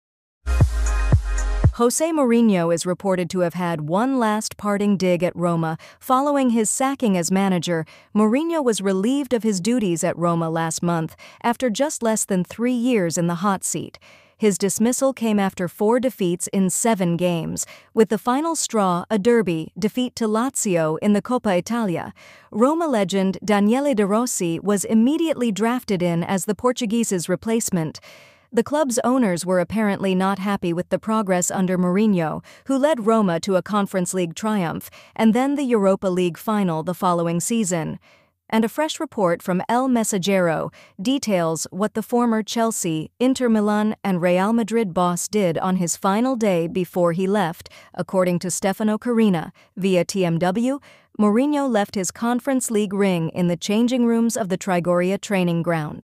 Jose Mourinho left one thing in Roma dressing room after brutal sacking and it speaks volumes
Jose Mourinho is reported to have had one last parting dig at Roma following his sacking as manager.

Mourinho was relieved of his duties at Roma last month after just less than three years in the hotseat.

His dismissal came after four defeats in seven games, with the final straw a derby defeat to Lazio in the Coppa Italia.Roma legend Daniele De Rossi was immediately drafted in as the Portuguese's replacement.

The club's owners were apparently not happy with the progress under Mourinho, who led Roma to a Conference League triumph and then the Europa League final the following season.

And a fresh report from l Messaggero details what the former Chelsea, Inter Milan and Real Madrid boss did on his final day before he left.

According to Stefano Carina, via TMW, Mourinho left his Conference League ring in the changing rooms of the Trigoria training ground.